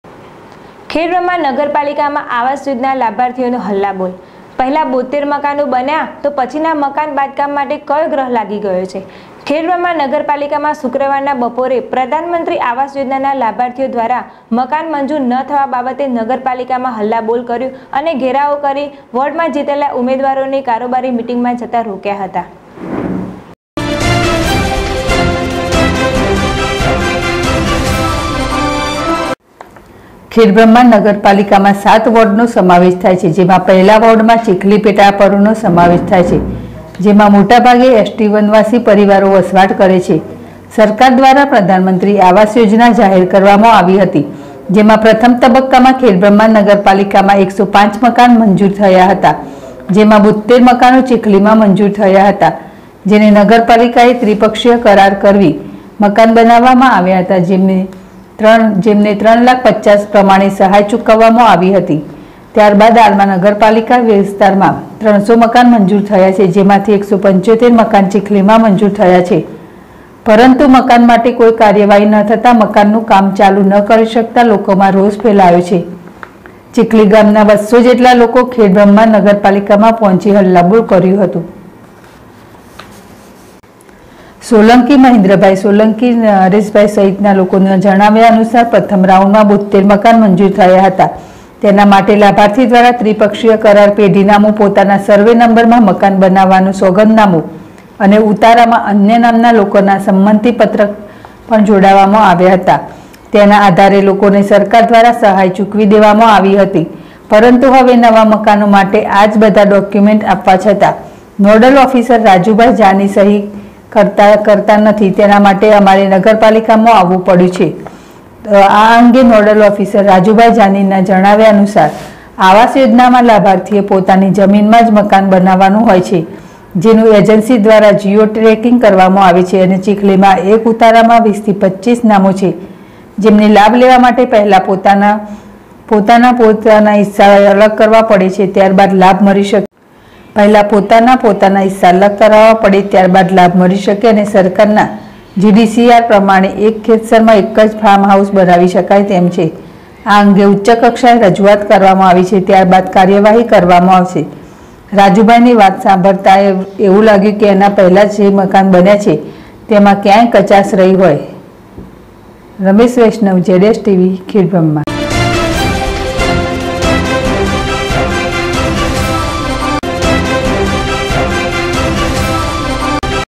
ખેડબ્રહ્મા નગરપાલિકામાં આવાસ યોજના લાભાર્થીઓનો હલ્લાબોલ
ખેડબ્રહ્મા નગરપાલિકામાં શુક્રવારના બપોરે પ્રધાનમંત્રી આવાસ યોજનાના લાભાર્થીઓ ઘ્વારા મકાન મંજુર ન થવા બાબતે નગરપાલિકામાં હલ્લાબોલ કર્યું હતું.